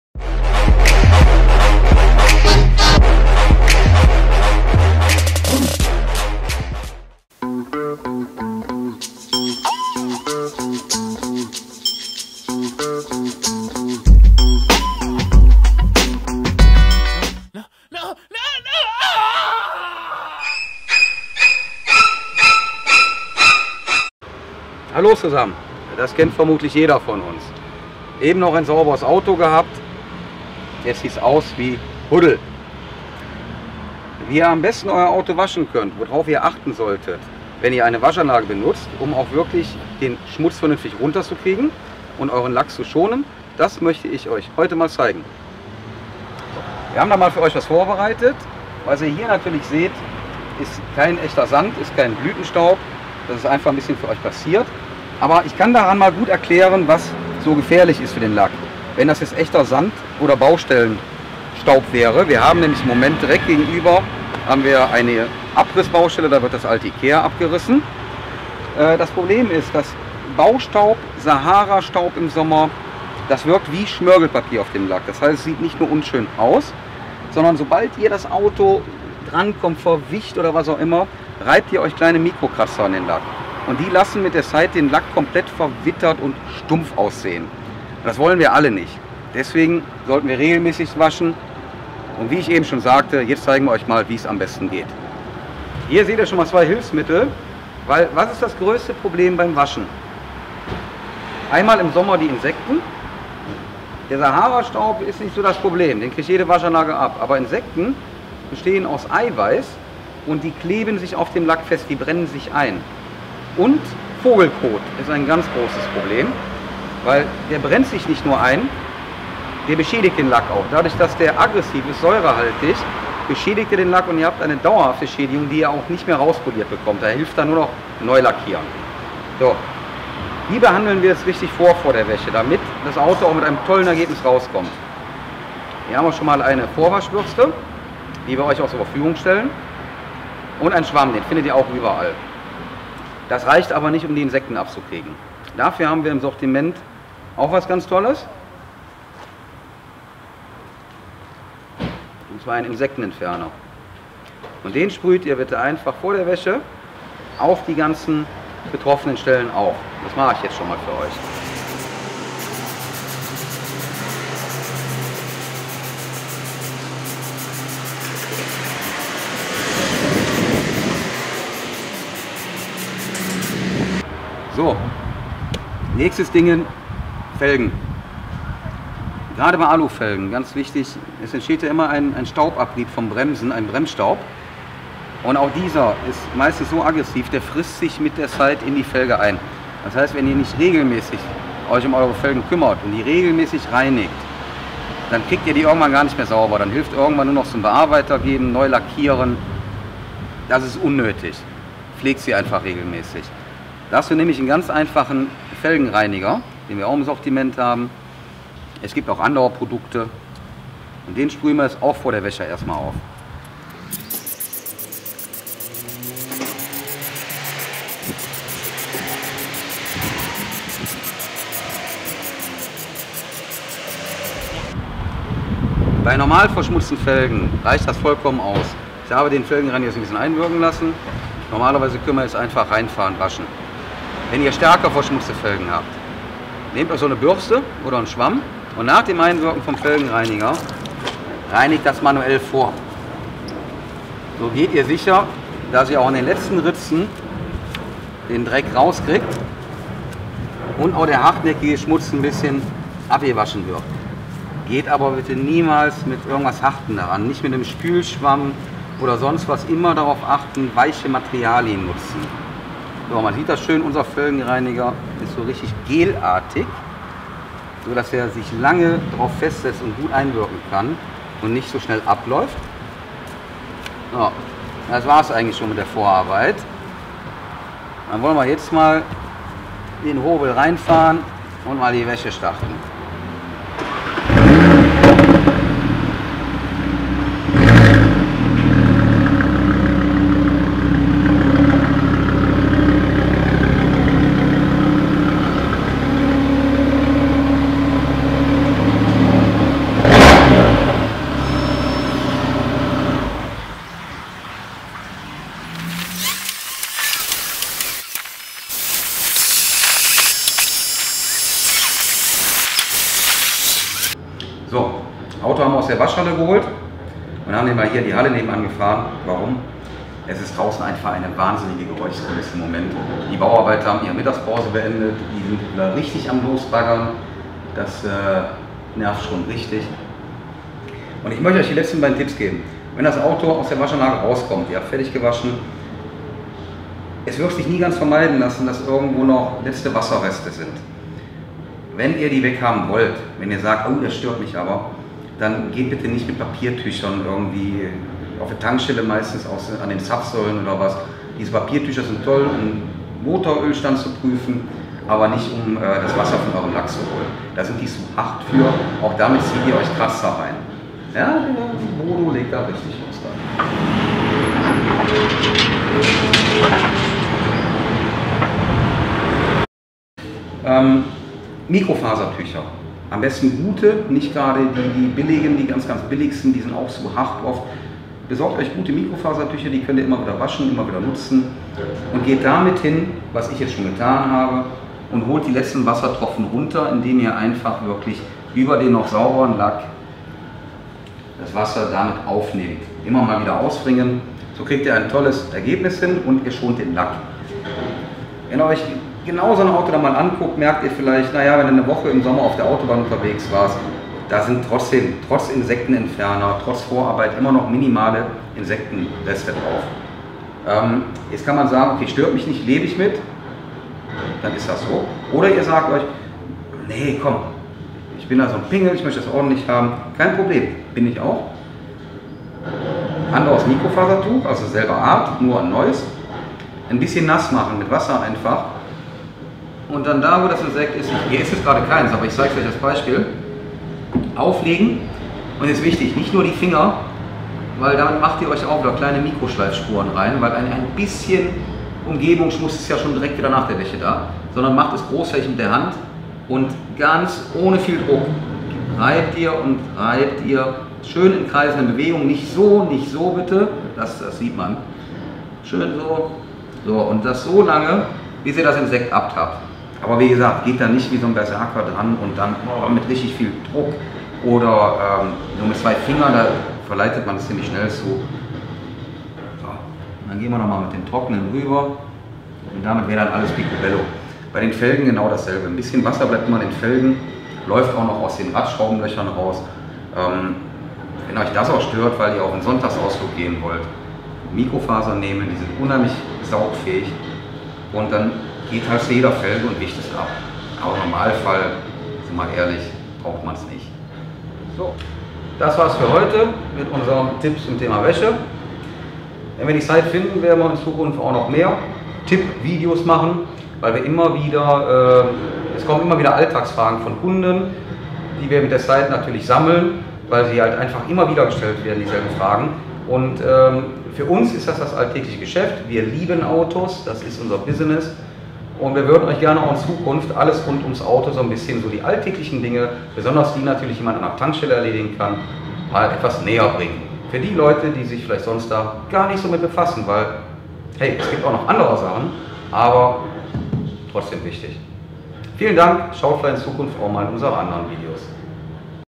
Fahrzeugvorbehandlung für die Wäsche
Hier zeigen wir euch, wie ihr euer Auto für die Maschinenwäsche richtig vorbehandelt. Verzeiht bitte die leichte Unschärfe, wir haben mit einer neuen Linse gearbeitet und vorher zu zu wenig Zeit zum Justieren gehabt. Wir geloben Besserung :)